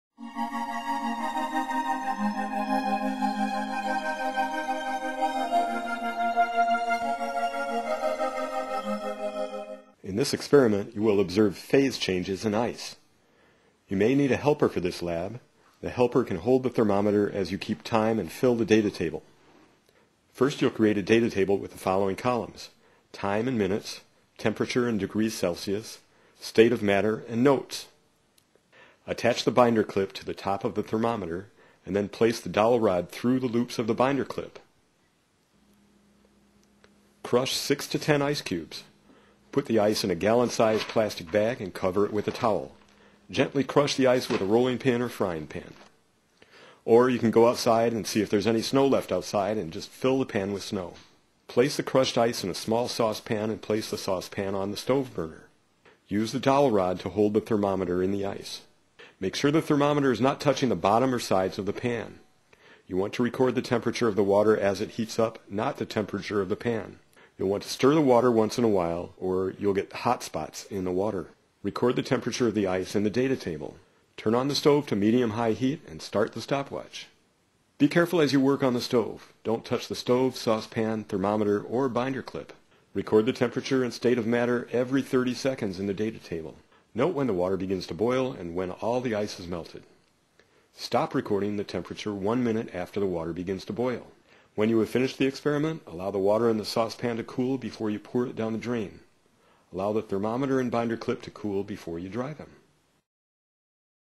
Phase Change Lab
How does temperature change as water goes through phase changes?